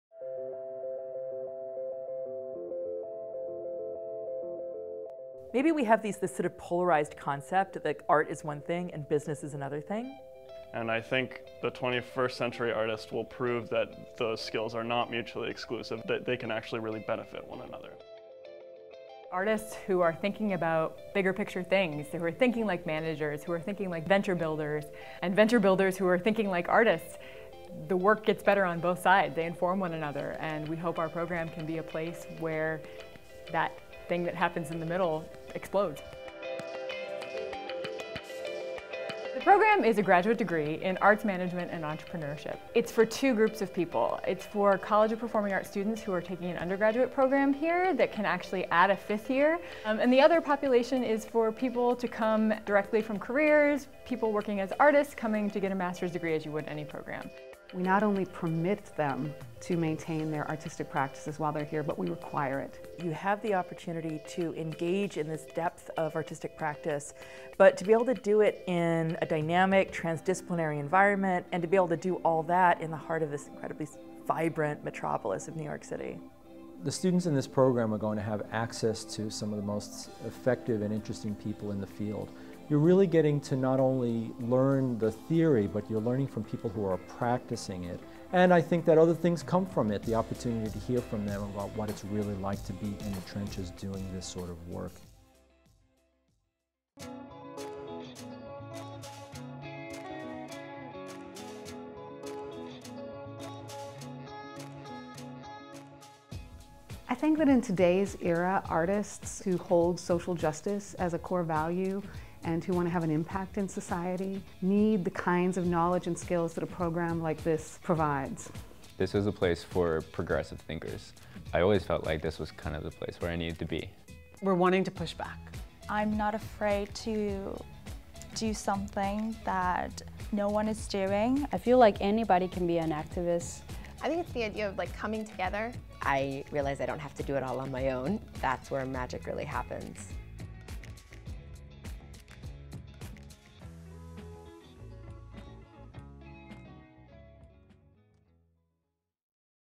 Arts Management and Entrepreneurship at The New School's College of Performing Arts
This video introduces a new, first-of-its-kind Master's program (MS in Arts Management and Entrepreneurship that encourages musicians, actors, dancers, choreographers, composers and other multidisciplinary artists to continue their artistic practices while developing enterprise skills.